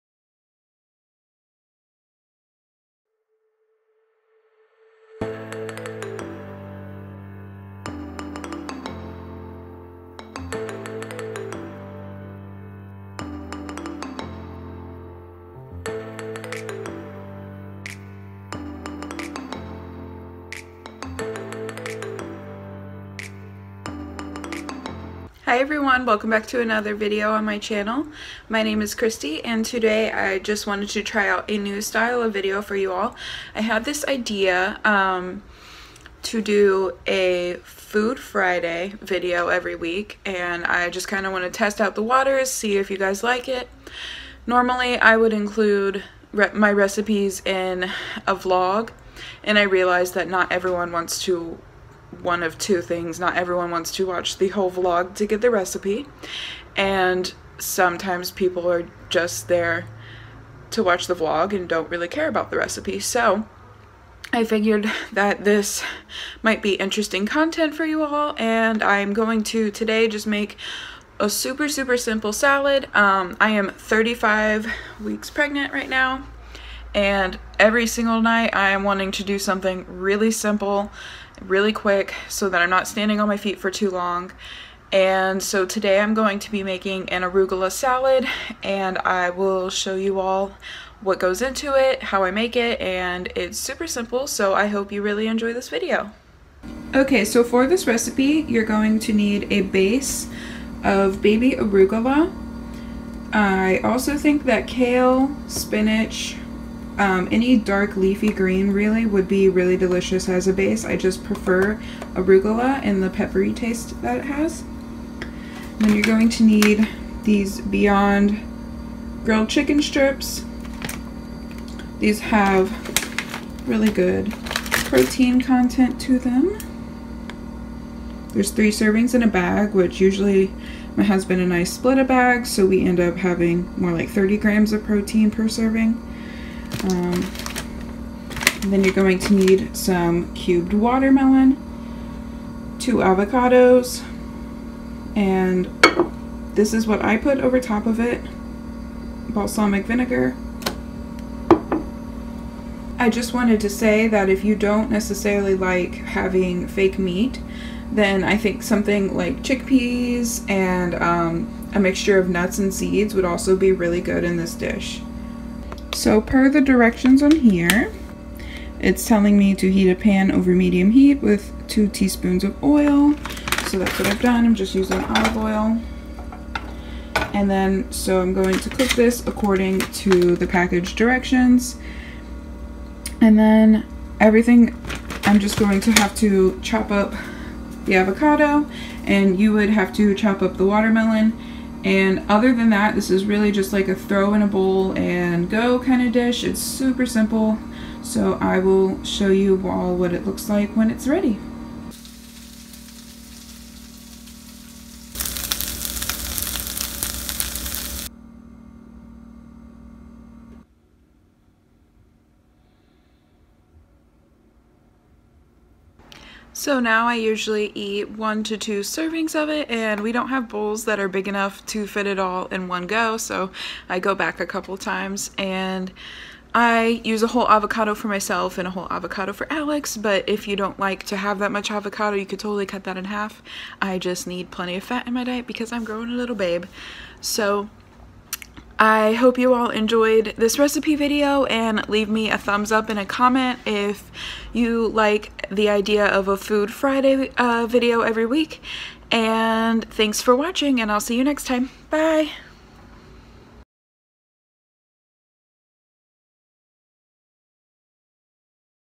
Vegan Chicken Arugula Salad - Food Friday - Easy Vegan Recipe
Hello and welcome back! I am testing out a new type of video, let me know what you think! Any recipes requests are greatly appreciated.